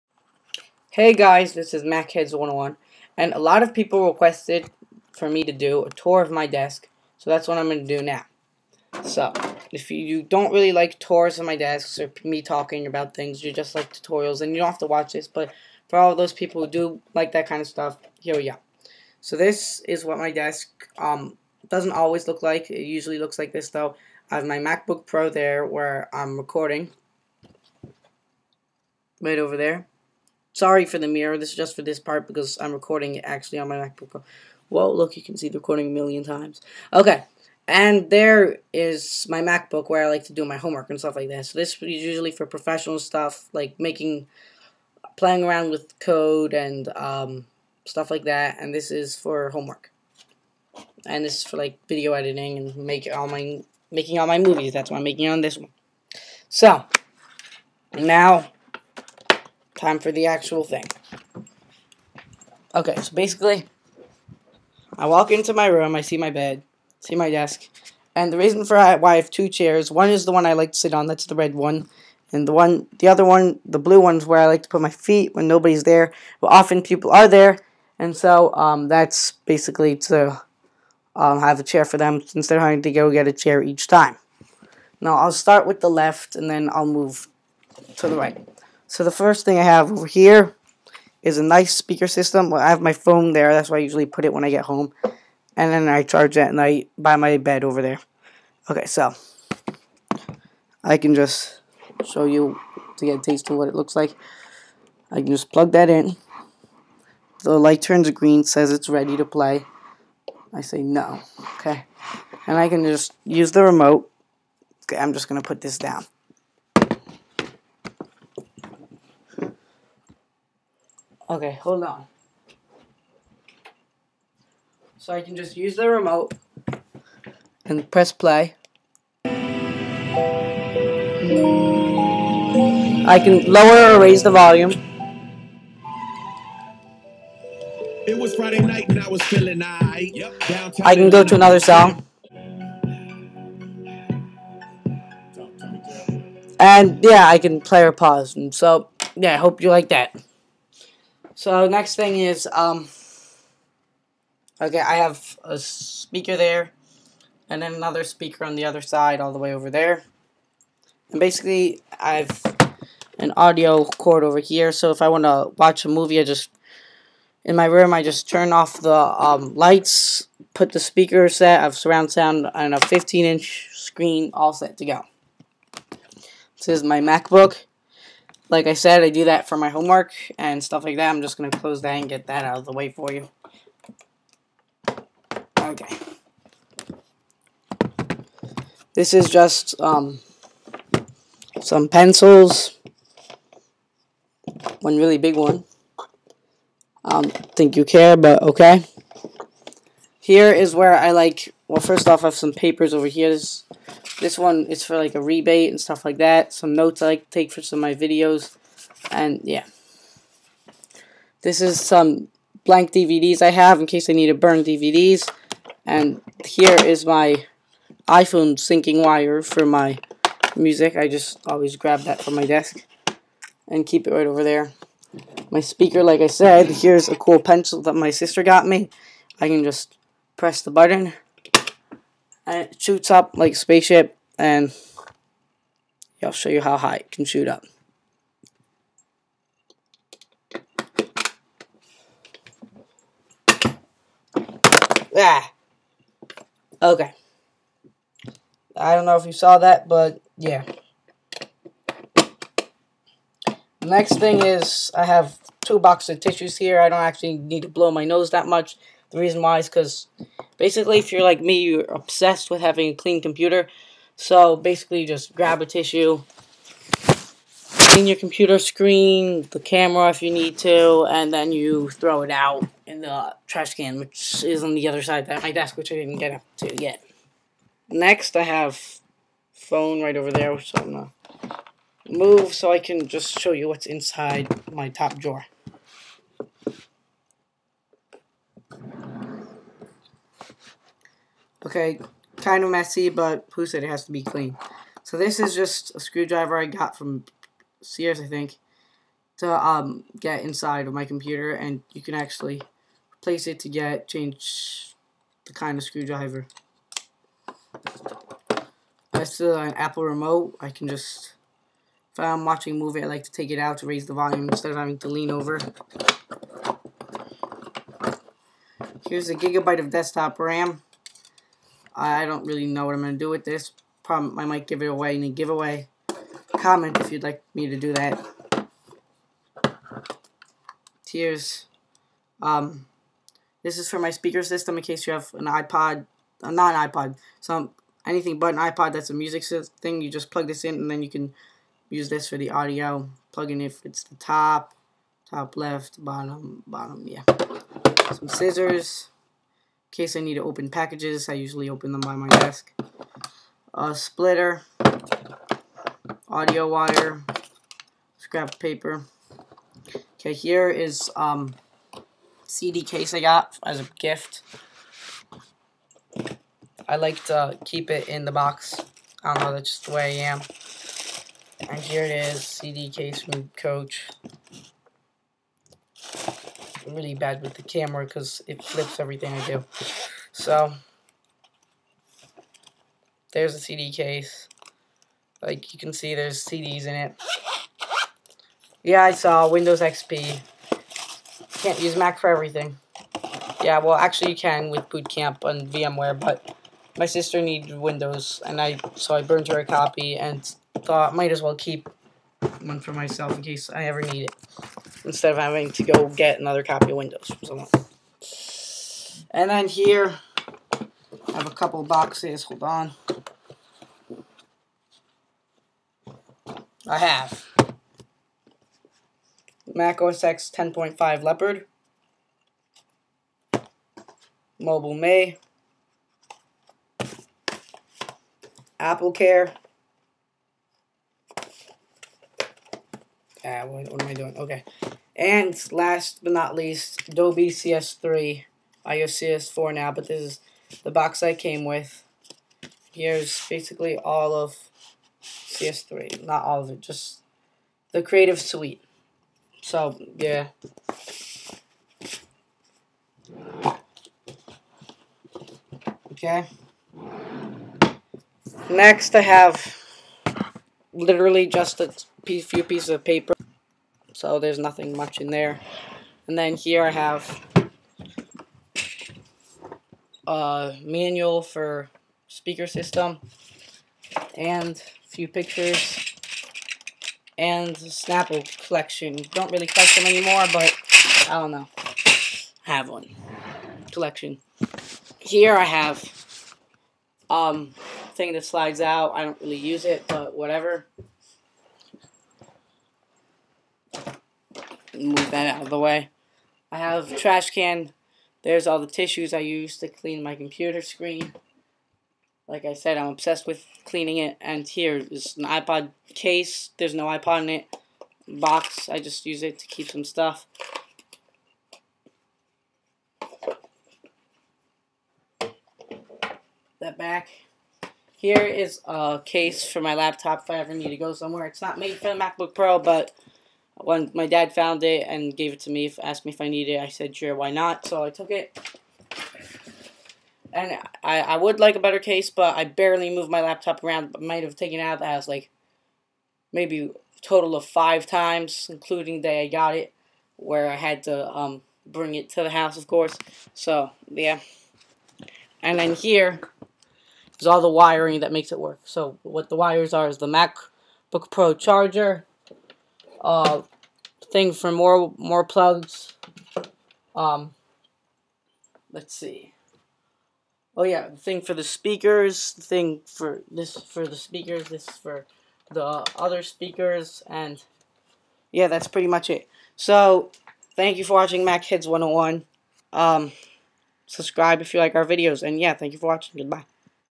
Tour of My Desk (Jon) 2/8/09
I show you a tour of my desk.